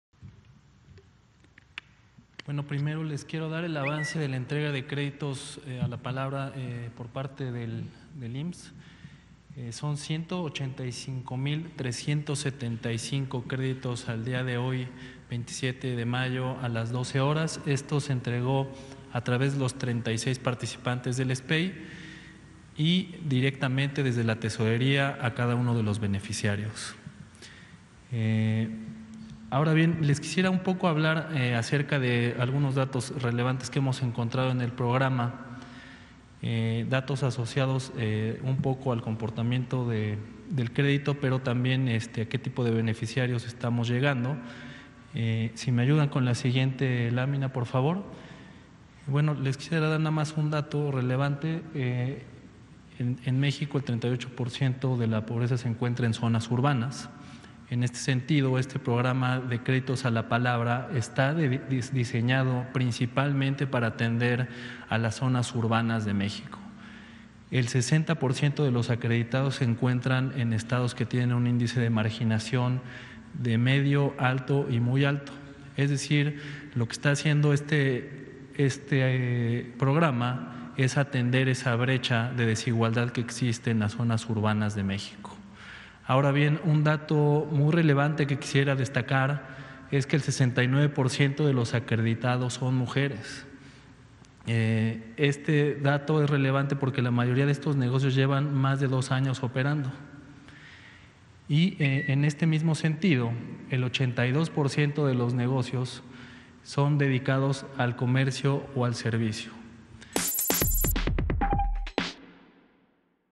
Se han entregado 185 mil 357 'Créditos Solidarios a la Palabra'
Omar Mejia, subtesorero de la Federación, presentó el avance en la entrega de los 'Créditos Solidarios a la Palabra', que al día de hoy acumula un total de 185 mil 357. El programa se creó para atender principalmente a las zonas urbanas.